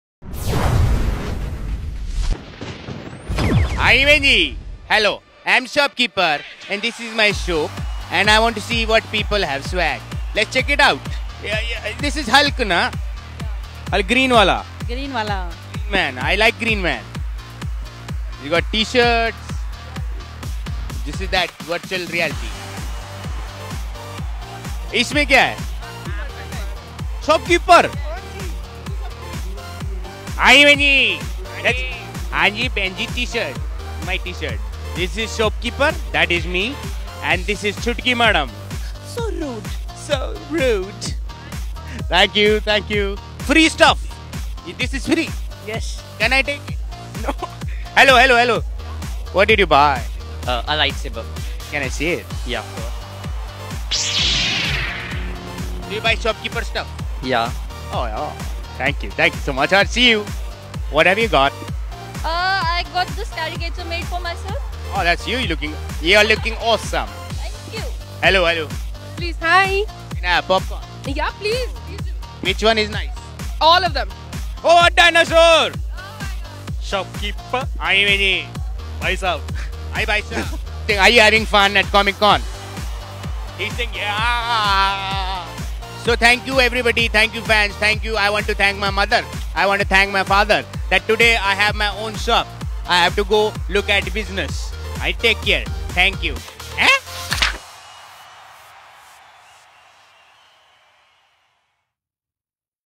What's In the Bag with Gaurav Gera at Alto Delhi Comic Con
Gaurav Gera aka Shopkeeper took on Alto Delhi Comic Con stage & asked the Visitors "What's In the Bag?"